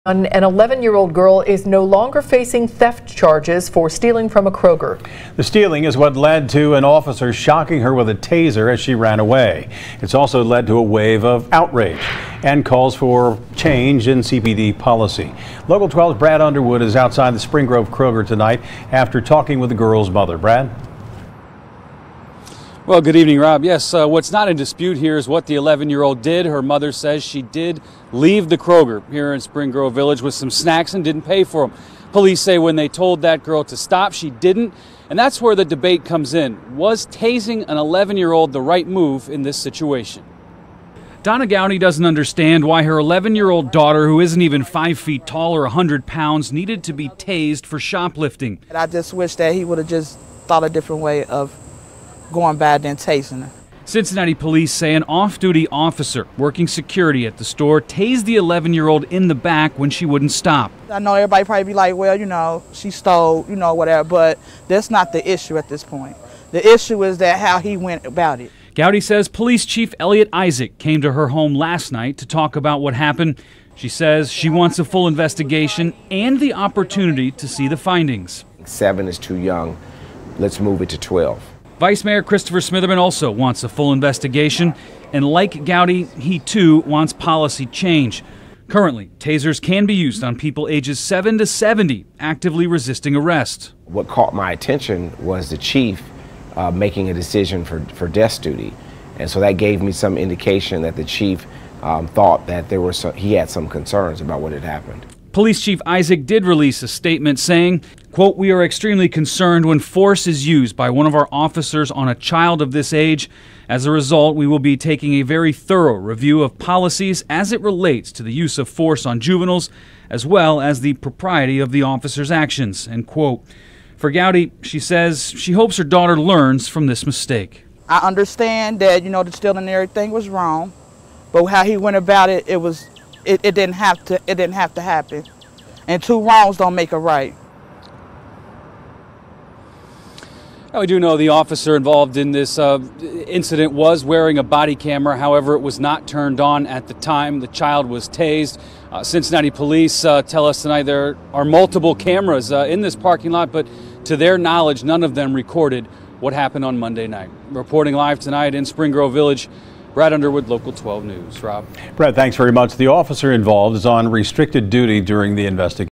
Mother of 11-year-old girl tased by police officer wants policy change
SPRING GROVE VILLAGE, Ohio (WKRC) - The mother of an 11-year-old girl tased by a police officer after a shoplifting incident at Kroger Monday says she wants the policy that allowed that to happen to be changed.

Hamilton County prosecutor Joe Deters is dropping the theft and obstructing official business charges against the girl. Mayor John Cranley released this statement Wednesday night:

Tasing an 11-year old who posed no danger to the police is wrong. I’m sorry for the harm to her and her family. This evening I called and asked Prosecutor Deters to drop charges against the girl. I’m happy to report that he did and I thank for him doing so.
What's not in dispute is what the 11-year-old did. She stole some snacks from the Spring Grove Village Kroger Monday night. Police say when she was told to stop she didn't, so she was tased, and that's where the difference of opinion comes in. Was the act of deploying a taser on the girl the right move?

Donna Gowdy doesn't understand why her daughter, who isn't even 5-feet tall or 100 pounds, needed to be tased for shoplifting.

"I just wish that he would have thought of a different way of going about it other than tasing her," Gowdy said.

Cincinnati Police say an off-duty officer working security at the store tased the 11-year-old in the back when she wouldn't stop.

"I know everybody probably be like, 'well, you know, she stole,' or whatever, but that's not the issue at this point. The issue is that how he went about it," she said.

Gowdy says Police Chief Eliot Isaac came to her home Tuesday night to talk about what happened. She says she wants a full investigation and the opportunity to see the findings.

Vice Mayor Christopher Smitherman also wants a full investigation, and, like Gowdy, he too wants the policy change. Currently, tasers can be used on people ages 7 to 70 actively resisting arrest.

"Seven is too young; let's move it to 12," said Smitherman. "What caught my attention was the chief making a decision for desk duty, and that gave me some indication that the chief thought there were some concerns about what had happened," Smitherman said.

Police Chief Isaac did release a statement saying:

We are extremely concerned when force is used by one of our officers on a child of this age. As a result we will be taking a very thorough review of our policies as it relates to using force on juveniles as well as the propriety of the officers actions.
For Gowdy, she says she hopes her daughter learns from this mistake.

"I understand that the stealing and everything was wrong, but how he went about it, it didn't have to happen, and two wrongs don't make a right," Gowdy says.